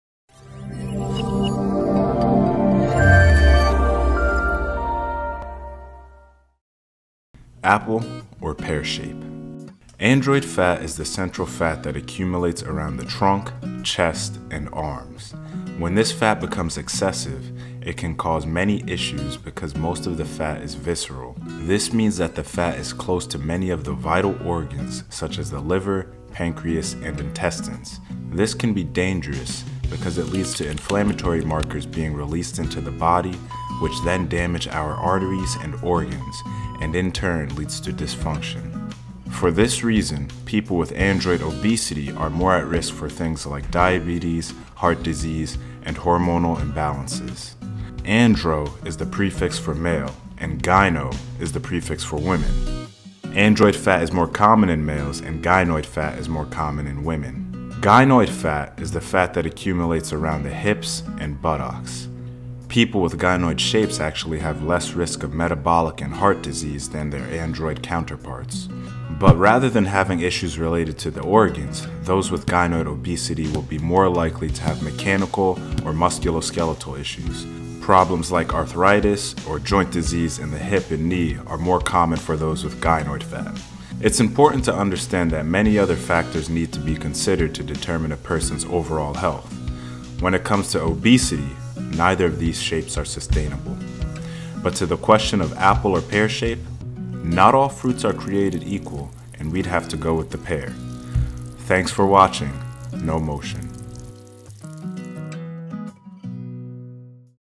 Body Types - Apple vs Pear Shape, Android and Gynoid Obesity
Apple or Pear shape?

“Android Fat” is the central fat that accumulates around the trunk, chest and arms.  When this fat becomes excessive, it can cause many issues, because most of the fat is visceral.  This means that the fat is close to many of the vital organs such as the liver, pancreas and intestines.  This can be dangerous because it leads to inflammatory markers being released into the body which then damage our arteries and organs, and in turn leads to dysfunction.  For this reason, people with Android obesity are more at risk for things like diabetes, heart disease, and hormonal imbalances.

“Andro” is the prefix for male, and “Gyno”  is the prefix for women.  Android Fat is more common in males, and Gynoid Fat is more common in women.

“Gynoid Fat” is the fat that accumulates around the hips and buttocks.   People with Gynoid shapes actually have less of a risk of metabolic and heart disease.  But rather than having issues related to the organs, those with Gynoid obesity will be more likely to have mechanical or musculoskeletal issues.  Problems like arthritis or joint disease in the hip and knee are more common for those with Gynoid fat.

It's important to understand that many other factors need to be considered to determine a person’s overall health.  When it comes to obesity, neither of these shapes are sustainable.  But to the question of apple or pear shape, not all fruits are created equal and we’d have to go with the pear.